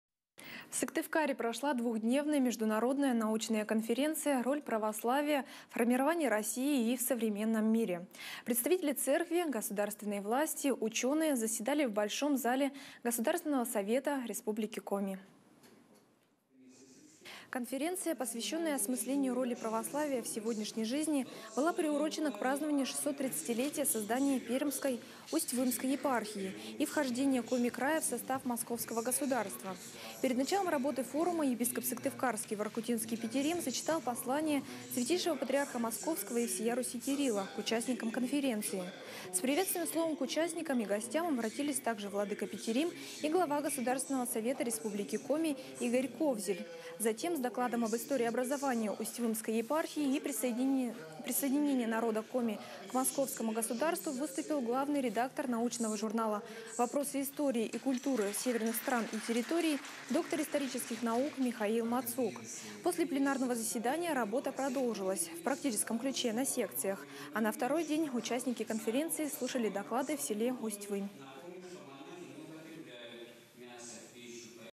Форум «Роль православия в формировании России»
В Сыктывкаре прошла двухдневная международная научная конференция «Роль православия в формировании России и в современном мире». Представители церкви, государственной власти и ученые заседали в Большом зале Государственного Совета Республики Коми.